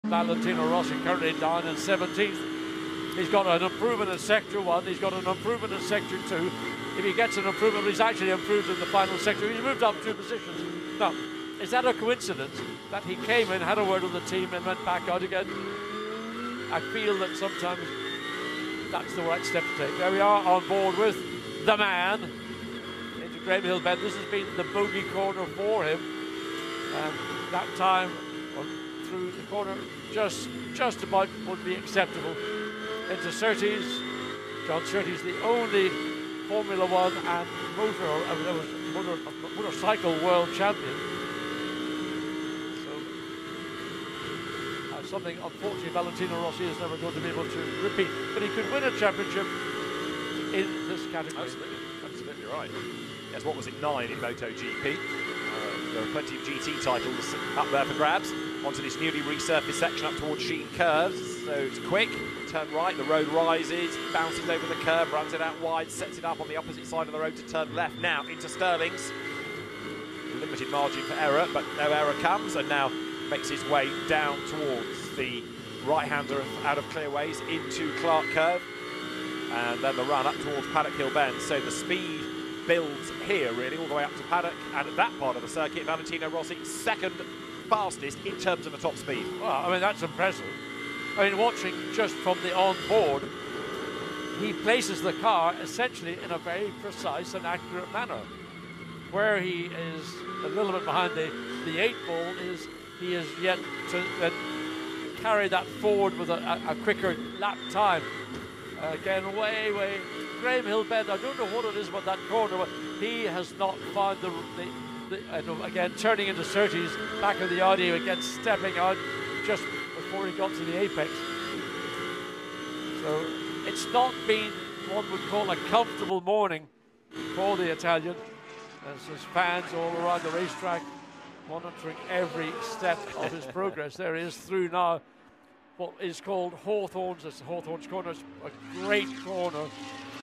Rossi Onboard Lap | GT World Challenge Brands Hatch 2022
9 time MotoGP champion Valentino Rossi moves into a new chapter of his career jumping into GT racing by competing in the 2022 GT World Challenge Europe this time getting his hands on the Brands Hatch circuit for the first time